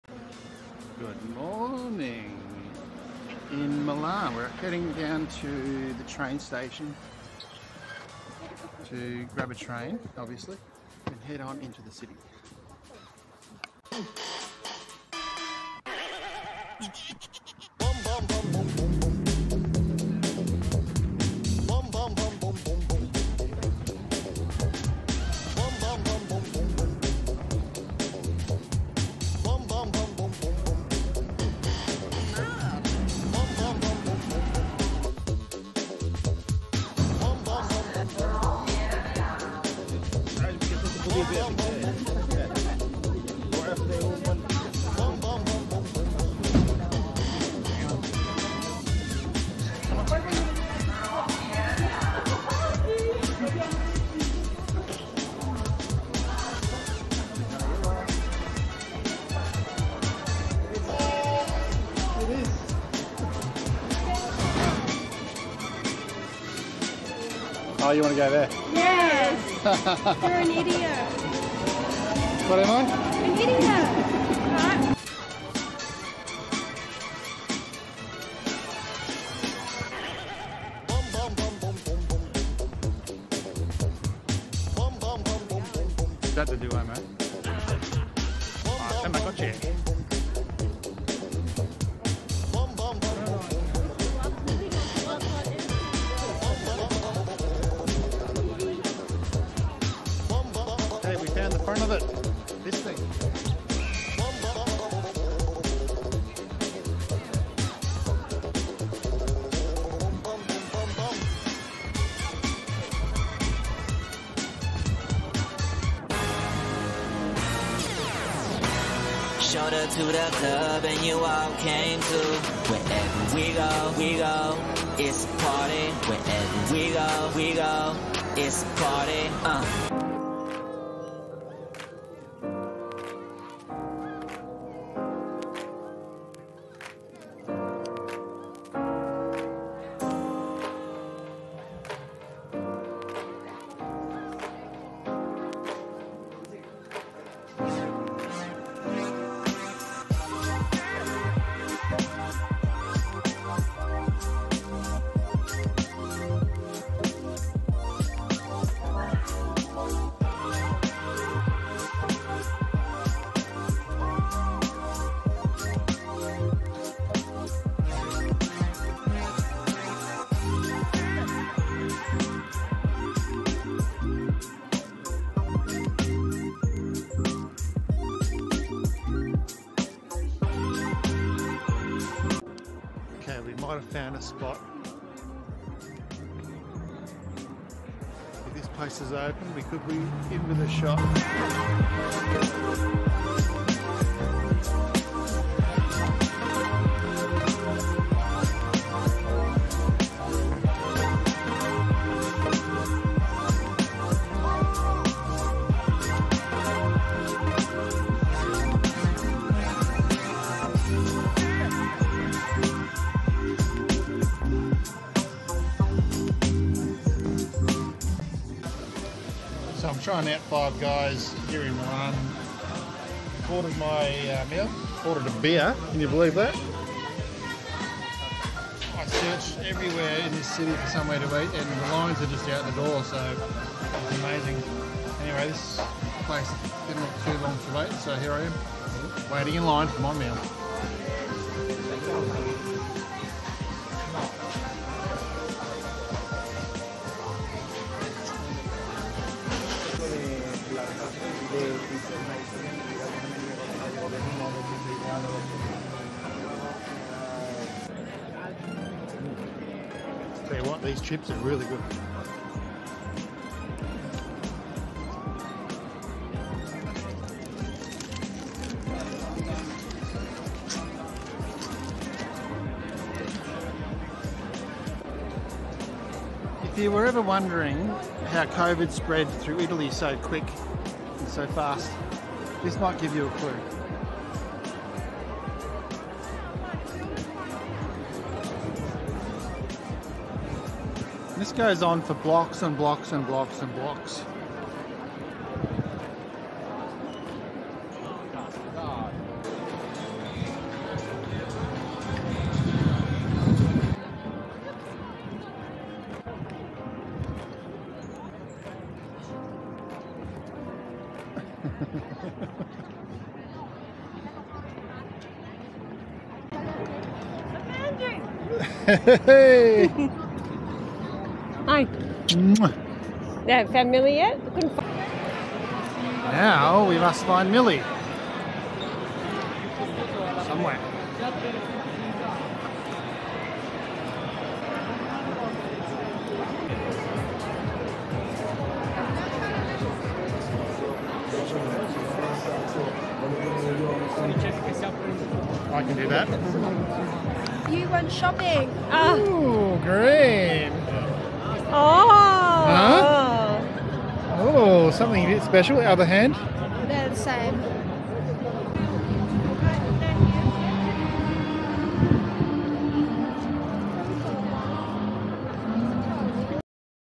ITALY | 15 | Milan is FULL-ON!!
So many people walking the streets of Milan. Doesn't anyone work around here? VLOGGING on the Insta360 Go2.